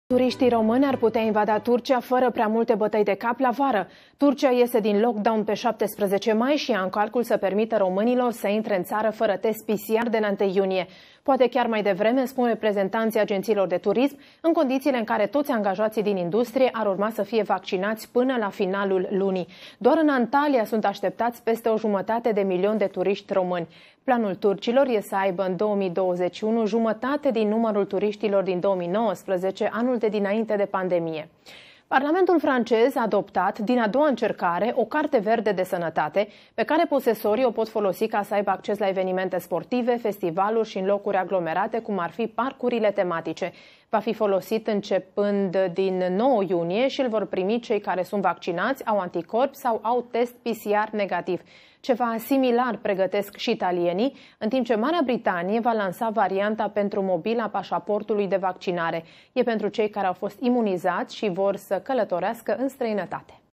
Turcia ar putea renunța la obligativitatea testelor PCR pentru turişti
➥ Ferit Turgut, preşedintele TURSAB, spune că după data de 17 mai nu vor mai cere obligativitatea prezentării unui rezultat negativ al testului PCR.
➥ „Până pe 17 mai este lockdown în Turcia, iar până atunci facem tot posibilul să se vaccineze cât mai multă lume”, a spus acesta.
➥ Preşedintele TURSAB adaugă faptul că după data de 17 mai, vor încerca să introducă un fel de listă galbenă, în funcţie de rata de infectare a fiecărei ţări.